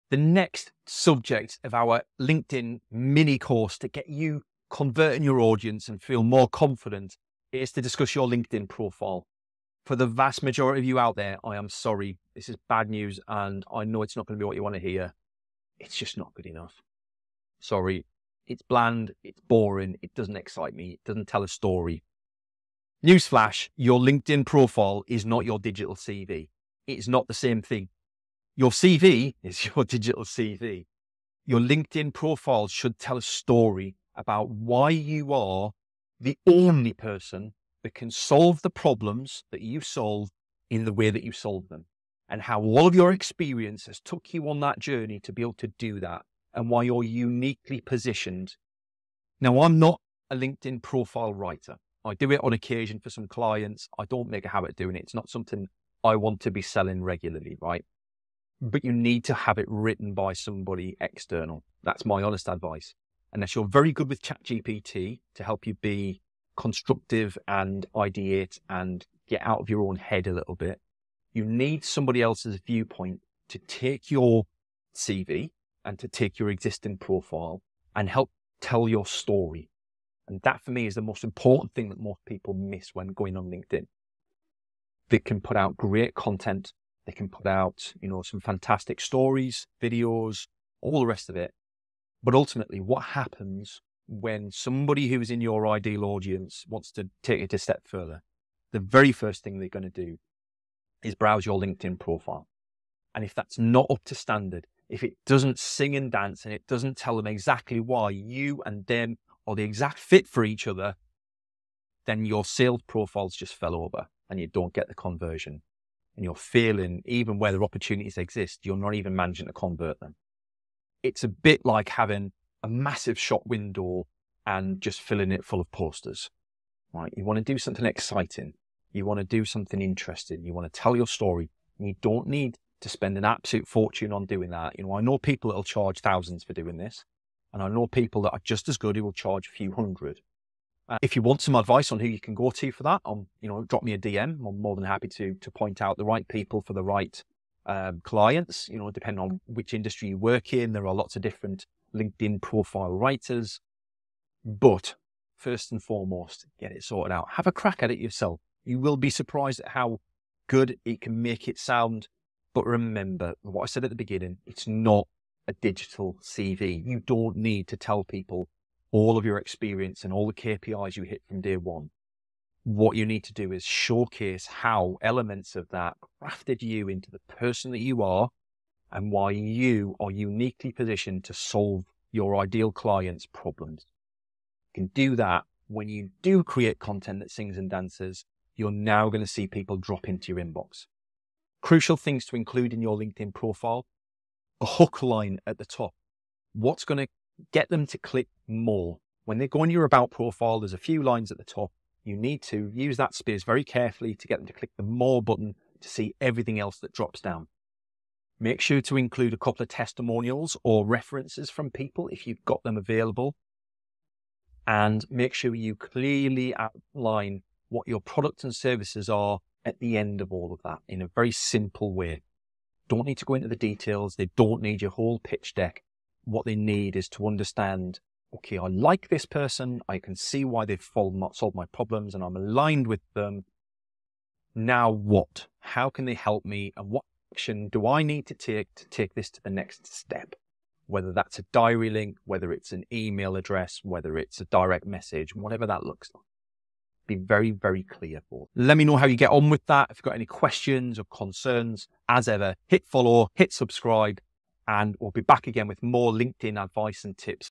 Reaching the Right Audience on LinkedIn - Stop Engaging the Wrong People 🎯
Are you reaching the right people on LinkedIn? In this video, I’ll help you stop wasting time engaging with the wrong audience and start attracting the clients who matter. If you're ready to transform your connections into genuine opportunities, you can’t afford to miss this.